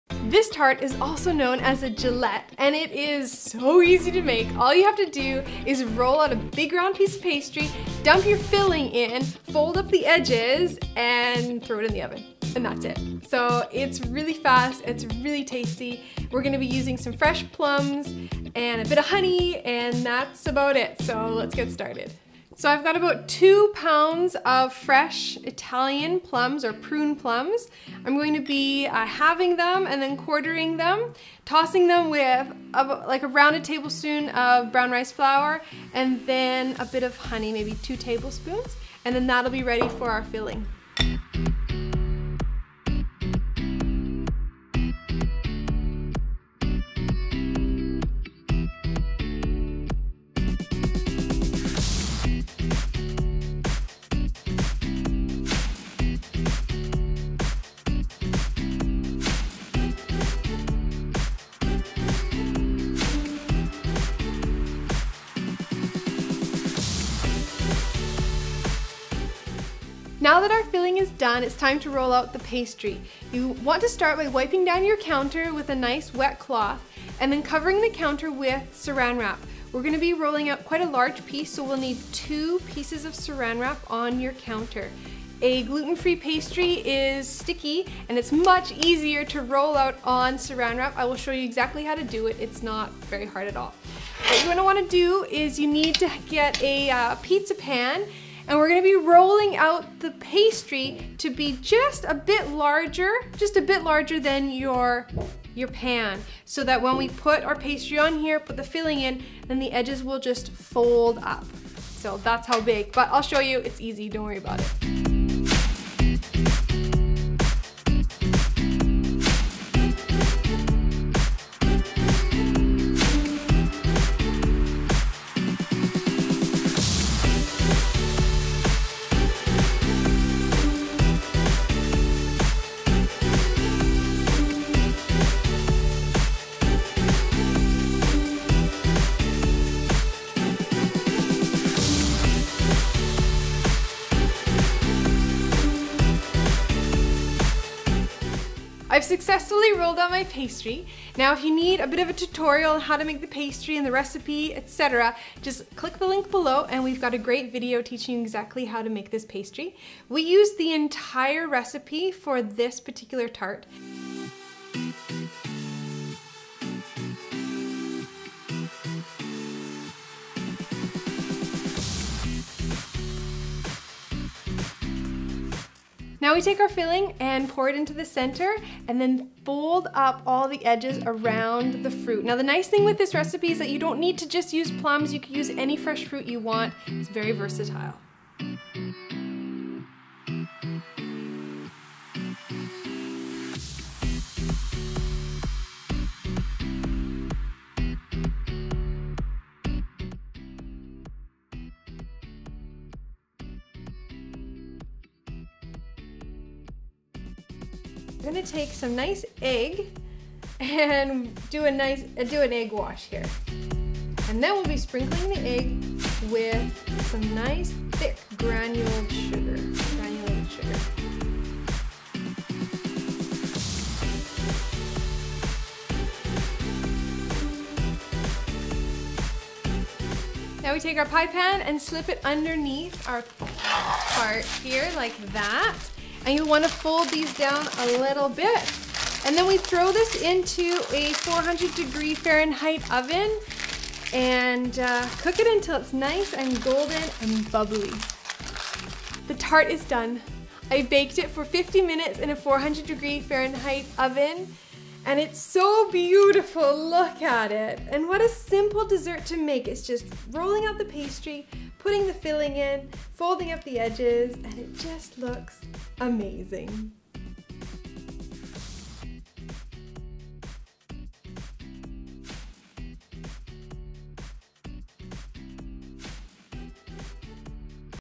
How To Make a Gluten Free Plum Tart
THIS RECIPE IS PART OF OUR FLAKY EBOOK SERIES.
To download your free copy of the Flaky ebook, sign-up to receive our regular blog posts.

Oh yeah. This plum tart is superb. Watch this 4 minute video to learn how to make it!